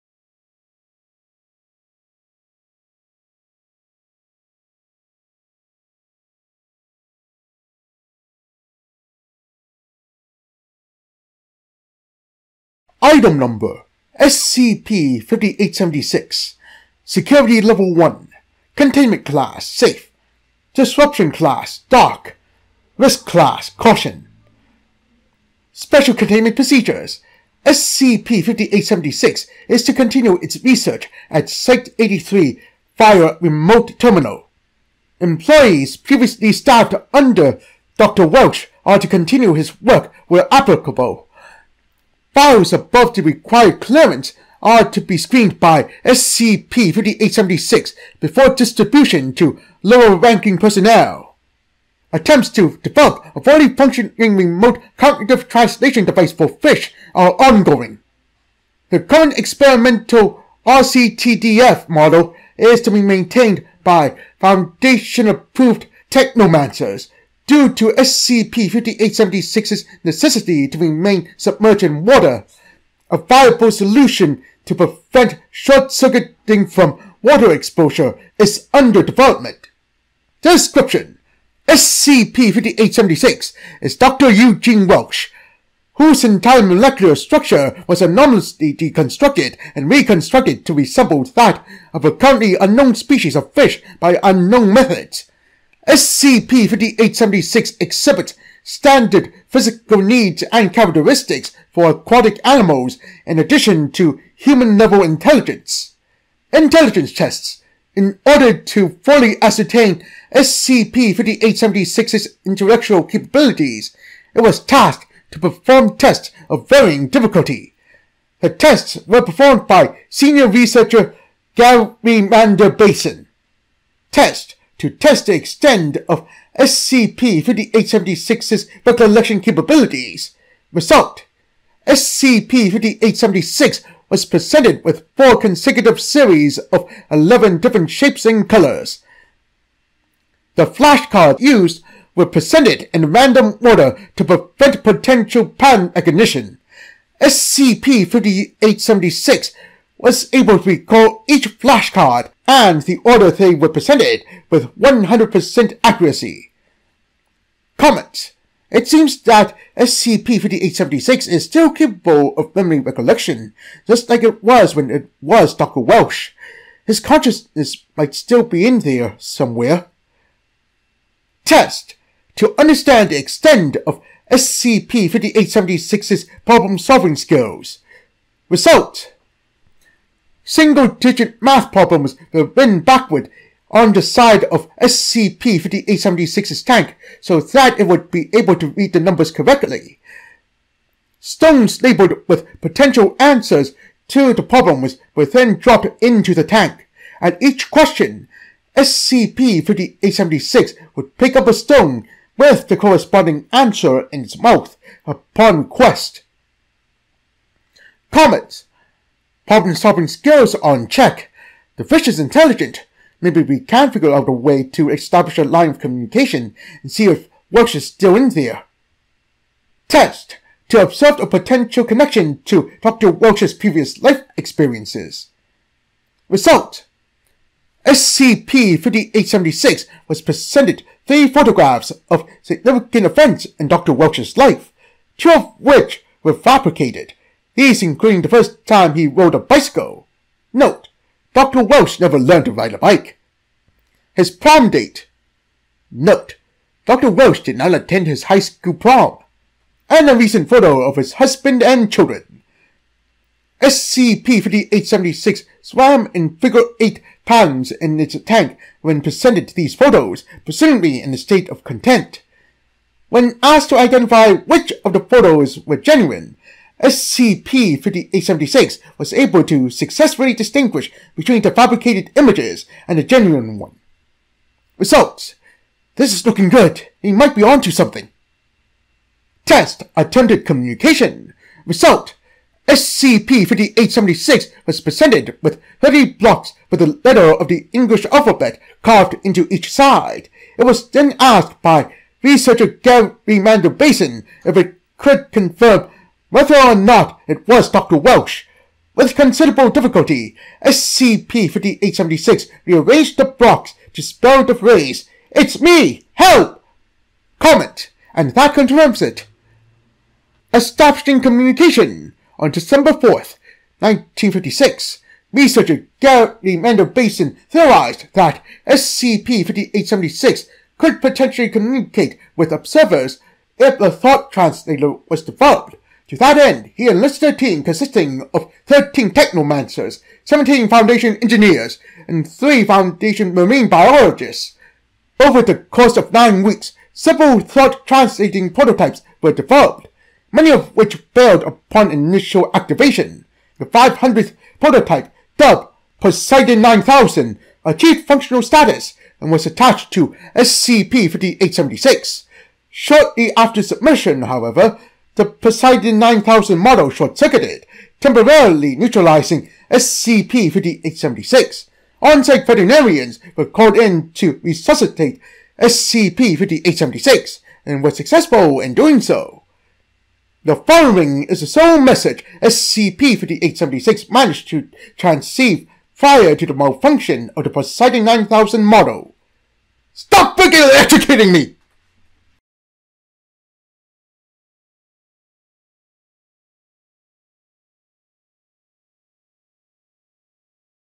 SCP 5876 Holy Mackeral
SCP-5876 is Foundation doctor, Doctor Eugene Welch, who was transformed into a fish with his intelligence and personality still intact. It should be noted that this SCP article takes place in an alternate timeline.